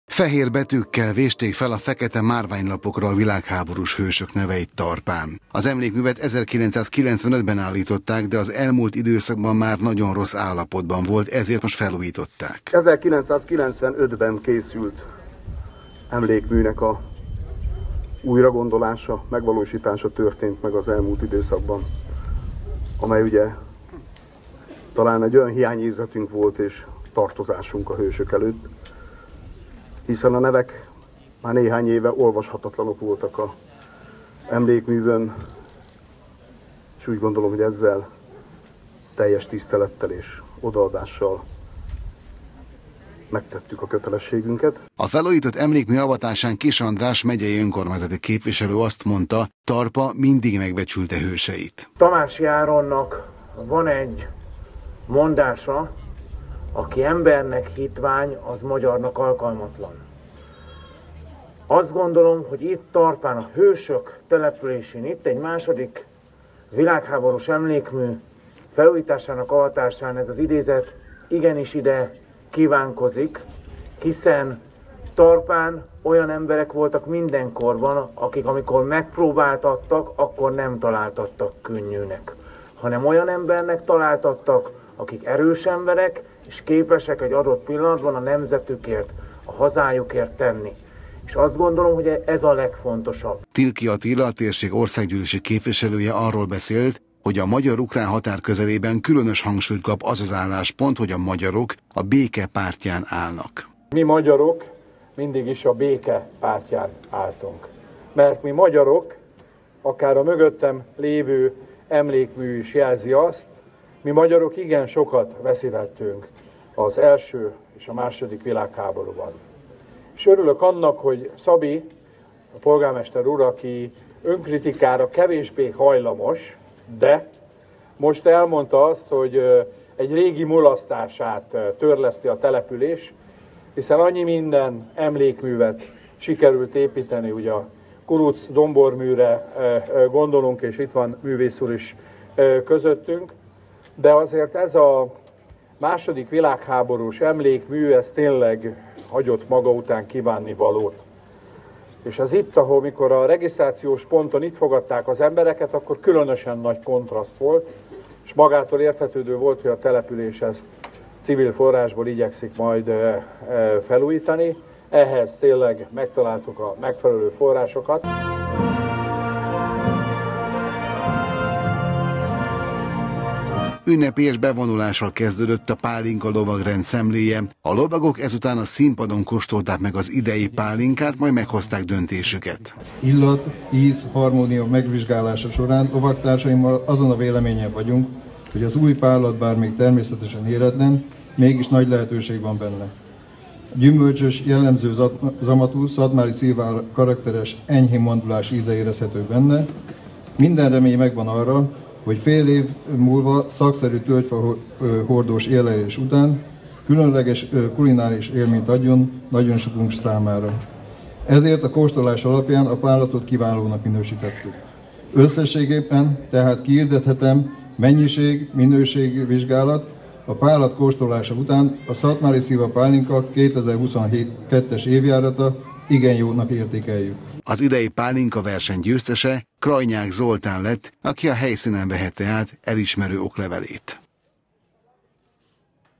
Tarpai szilvanapok
Felújított emlékmű előtt tisztelegtek elhunyt háborús hősök előtt Tarpán. A megemlékezés része volt a településen szervezett Szilva napnak. Nemcsak a régmúlt hősei előtt hajtottak fejet, de a jelen legjobbjait is díjazták. A Pálinka lovagrend idén is megválasztotta az év legjobb szatmári szilvapálinkáját és díjazta annak készítőjét.